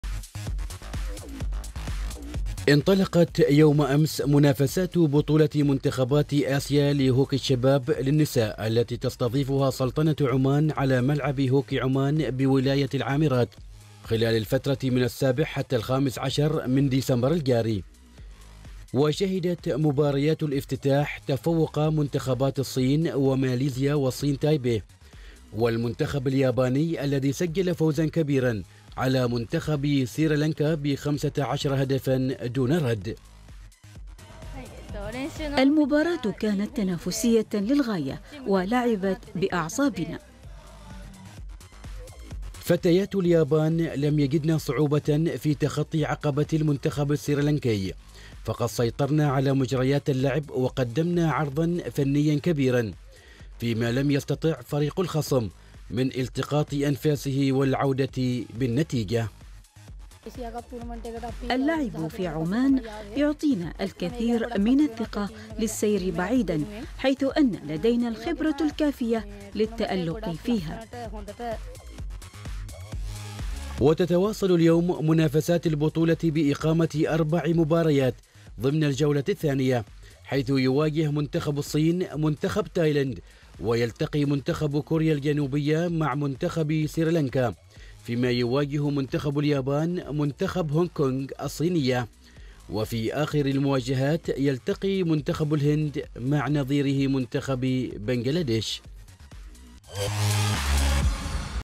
إثارة كبيرة تشهدها منافسات اليوم الأول لبطولة آسيا لهوكي النساء
تقرير : أحمد العجمي
تصوير : أحمد بني عرابه